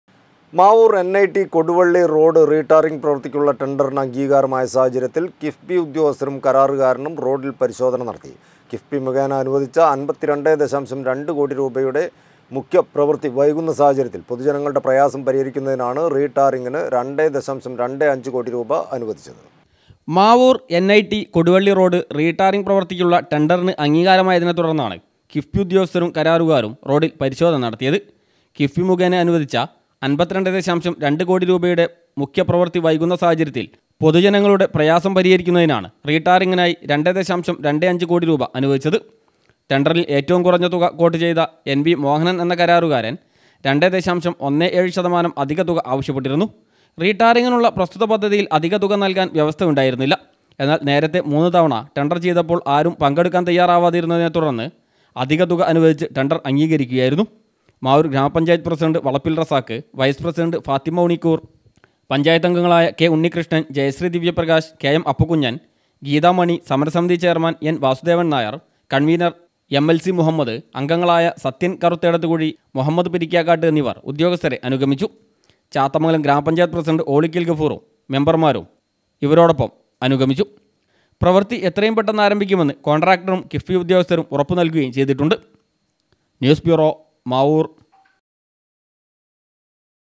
മാവൂർ എൻ.ഐ.ടി കൊടുവള്ളി റോഡ് റീ ടാറിങ് പ്രവൃത്തിക്കുള്ള ടെൻഡറിന് അംഗീകാരമായ സാഹചര്യത്തിൽ
മാവൂർ എൻ.ഐ.ടി കൊടുവള്ളി റോഡ് റീ ടാറിങ് പ്രവൃത്തിക്കുള്ള ടെൻഡറിന് അംഗീകാരമായ സാഹചര്യത്തിൽ  കിഫ്ബി ഉദ്യോഗസ്ഥരും കരാറുകാരനും റോഡിൽ പരിശോധന നടത്തി.